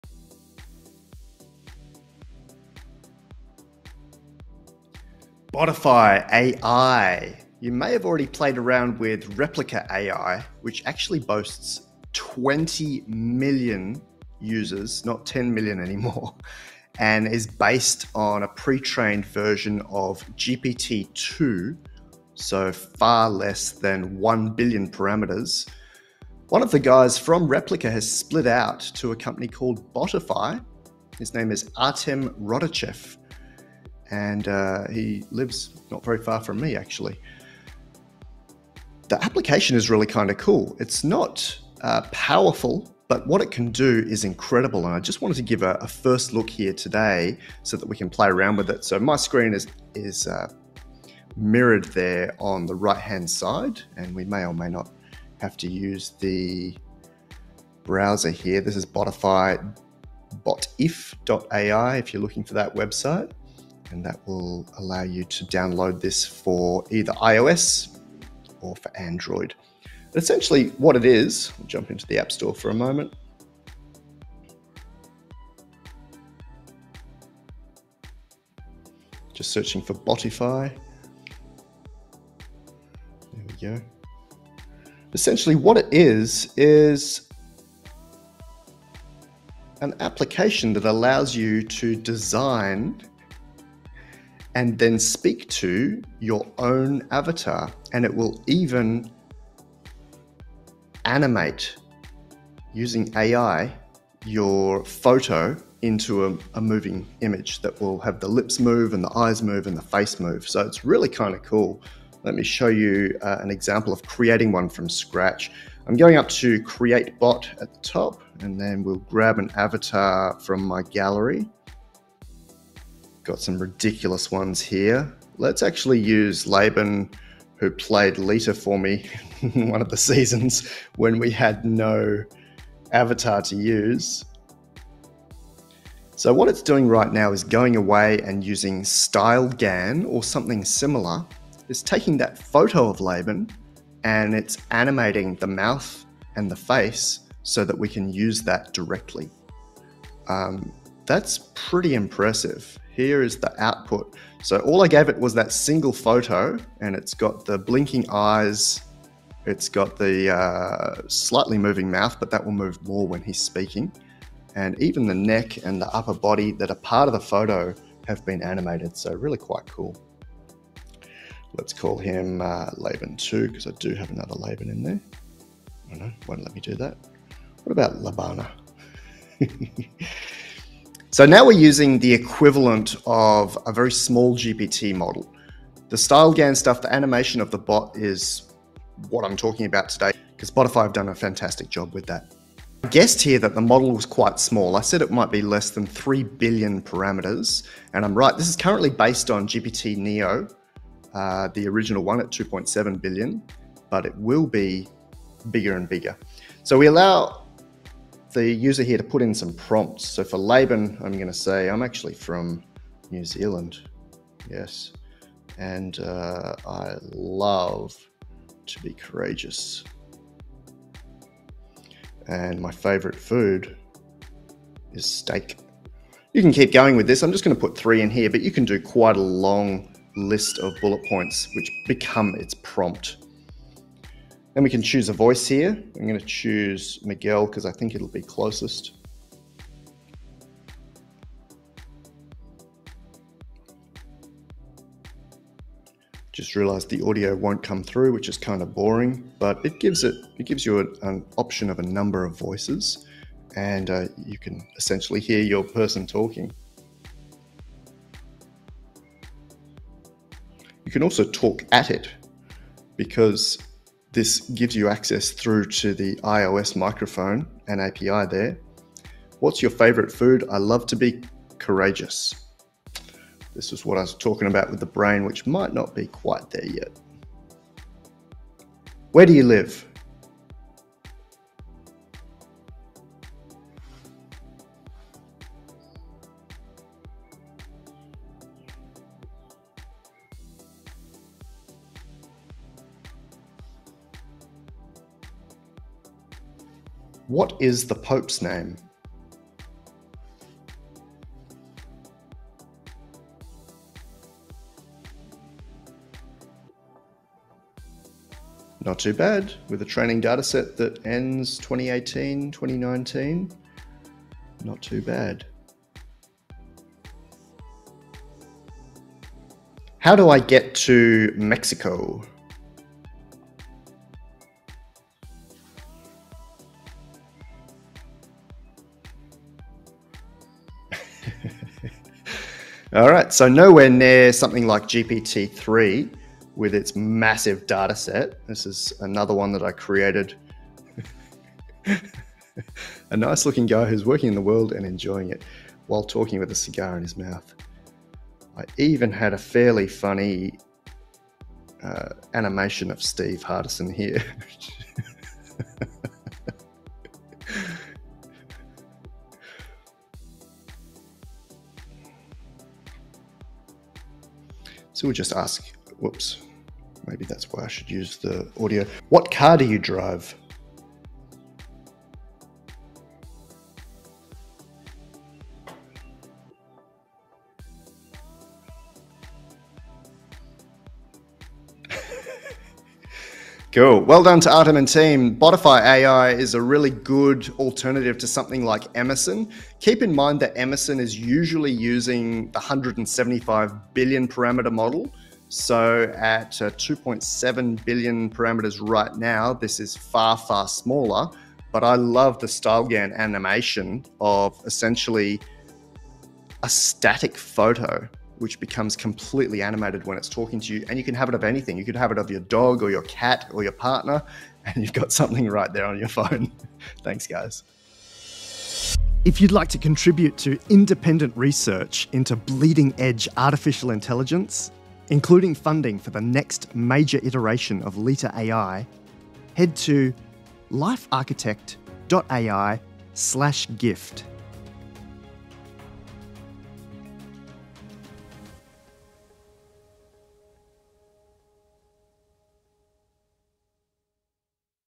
First look - Botify (2.7B+) - chatbot, similar to Quickchat.ai Emerson, Launched Nov/2021
Dr Alan D. Thompson is a world expert in artificial intelligence (AI), specialising in the augmentation of human intelligence, and advancing the evolution of ‘integrated AI’.

Under licence.